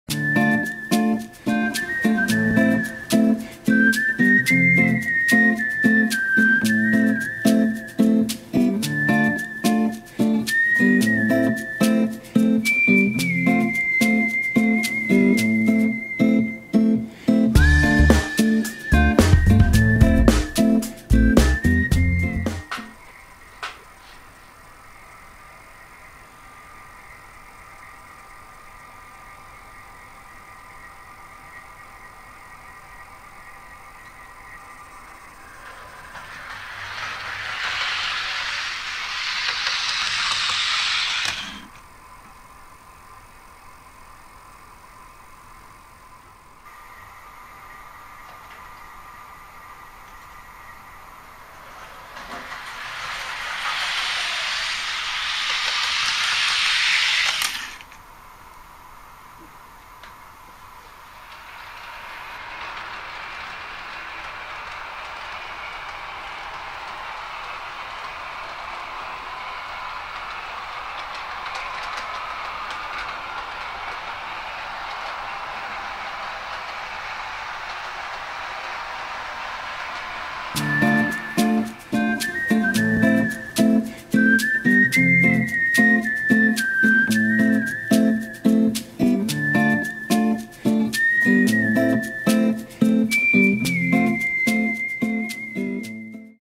Mittelstadt-TV, Ablauf am Ablaufberg auf einen Hemmschuh (Funktionstest)
Heute gibt es einen Funktionstest eines Weinert Hemmschuhs der bearbeitet wurde. Hier kann man mal sehen was in H0 alles möglich ist. Viel Spaß beim anschauen.